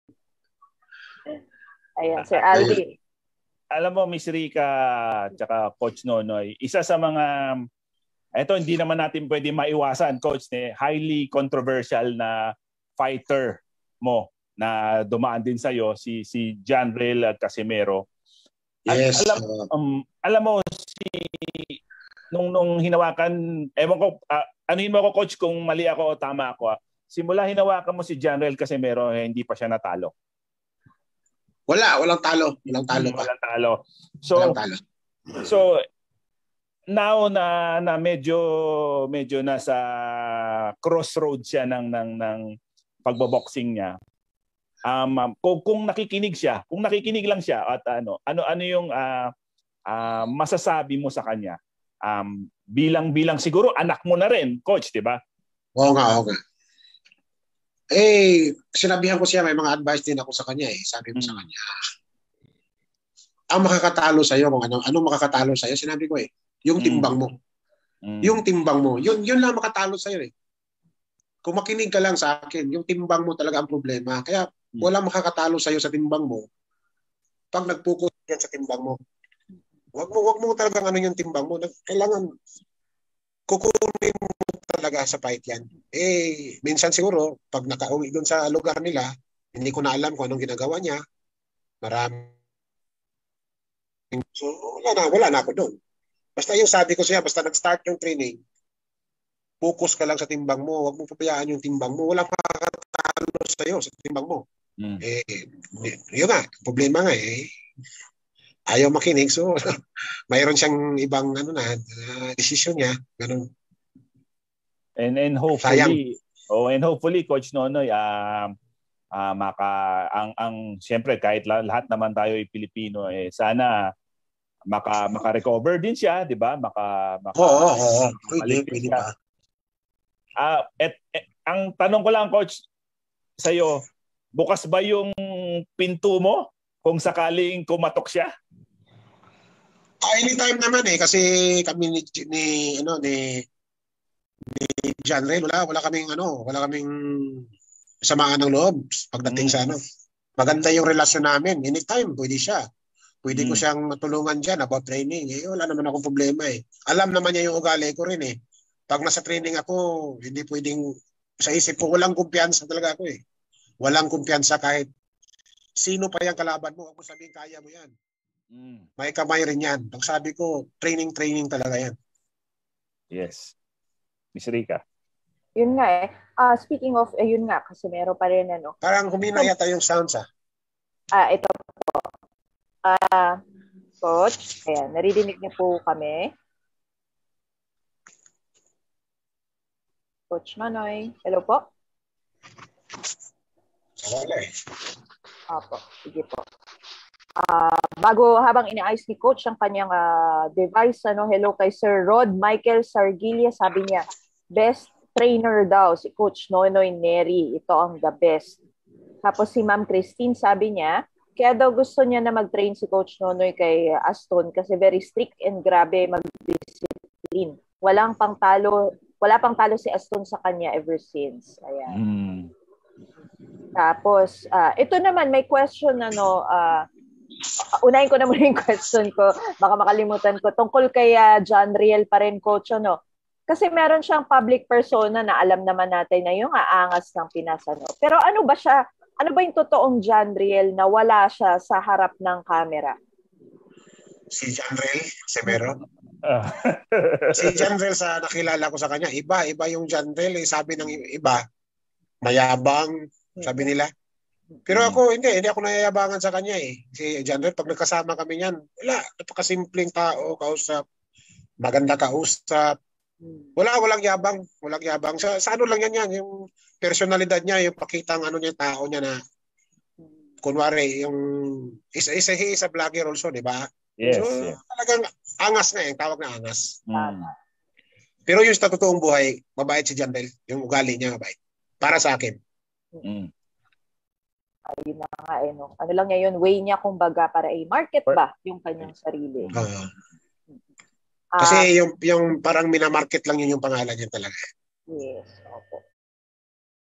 Anong masasabi ni Coach Nonoy Neri tungkol kay Quadro Alas, John Riel Casimero 🇵🇭🥊
Bilang kaibigan at dati nyang boxer, anong masasabi ni Coach Nonoy Neri tungkol kay Quadro Alas, John Riel Casimero 🇵🇭🥊